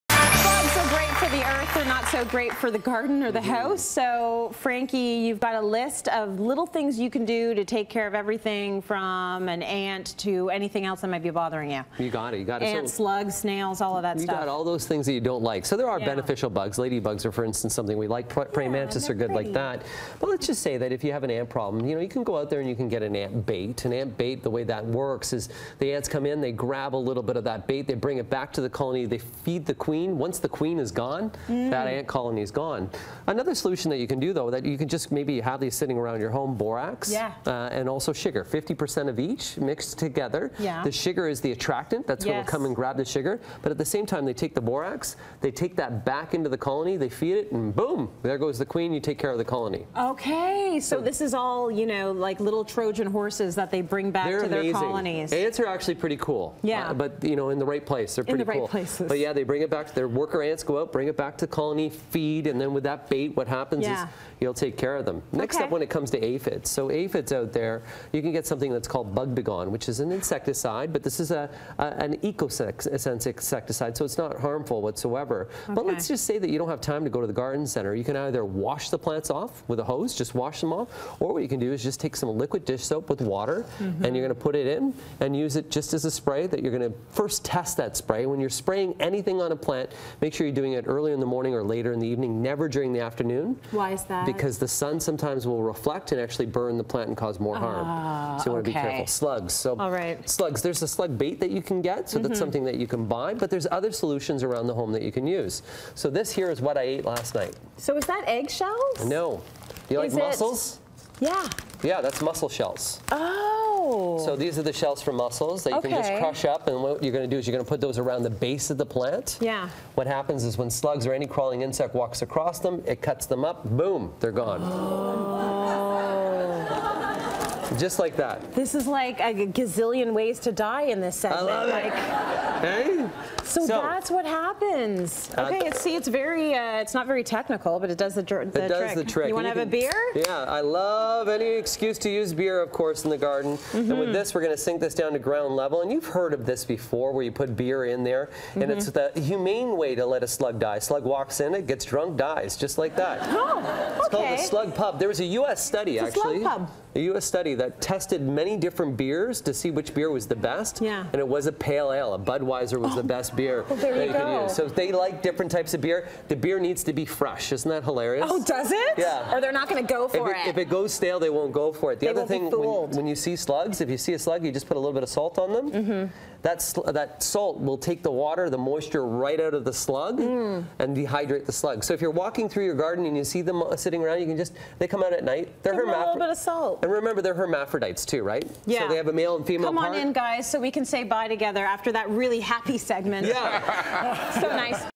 How to get bugs out of your garden and home
Have a bug problem? Gardening expert Frank Ferragine shares different ways get rid of bugs in your garden and home for good.